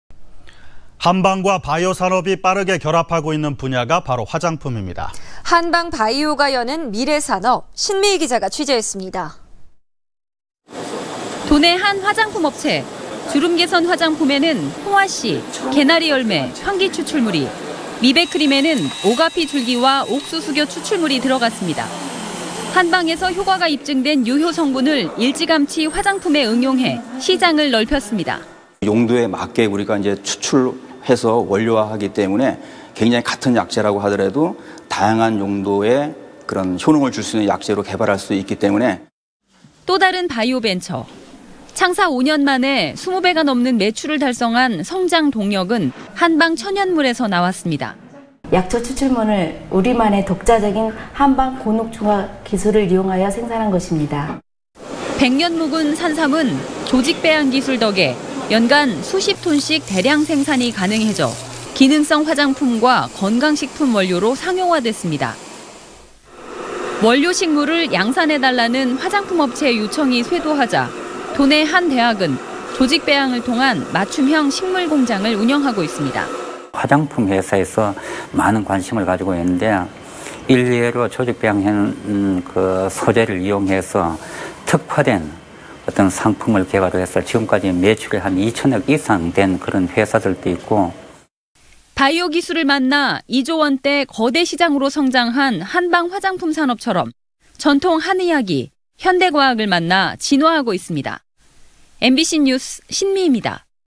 [청주MBC뉴스] 한방+바이오가 여는 미래산업
◀앵커▶
 한방과 바이오산업이 빠르게 결합되고 있는
분야가 바로 화장품입니다.  한방바이오가
여는 미래산업, 신미이 기자가 취재했습니다.
◀ＥＮＤ▶

 도내 한 화장품 업체,

 주름 개선 화장품에는
홍화씨, 개나리 열매, 황기 추출물이,
미백 크림에는 오가피 줄기와 옥수수겨
추출물이 들어갔습니다.

 한방에서 효과가 입증된 유효성분을
일찌감치 화장품에 응용해,
시장을 넓혔습니다.

         ◀ＩＮＴ▶
윤경섭 연구소장/사임당화장품

 또다른 바이오벤처,

 창사 5년만에 20배가 넘는 매출을 달성한
성장 동력은 한방 천연물에서 나왔습니다

      ◀ＩＮＴ▶
  이정옥 본부장/ 아리바이오

 100년 묵은 산삼은 조직 배양기술 덕에
연간 수십톤씩 대량 생산이 가능해져,
기능성 화장품과 건강식품 원료로
상용화 됐습니다.

-화면전환-

 원료식물을 양산해 달라는
화장품 업체의 요청이 쇄도하자,
도내 한 대학은 조직배양을 통한
맞춤형 식물공장을 운영하고 있습니다.

        ◀ＩＮＴ▶
백기엽 교수/충북대 원예학과

 바이오산업을 만나 2조 원대 거대 시장으로
성장한 한방화장품 산업처럼,
전통한의학이 현대과학을 만나
진화하고 있습니다. MBC 뉴스 신미입니다.